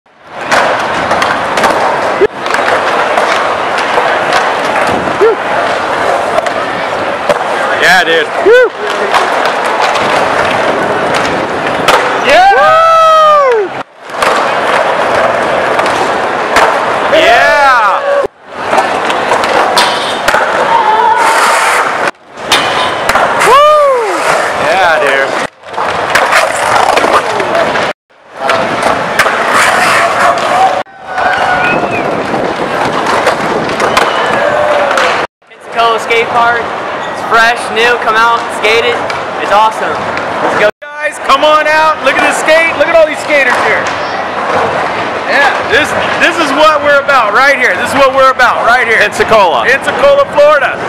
Opening day Pensacola Skate Park
Great opening day at the Blake Doyle Skate Park in Pensacola Florida.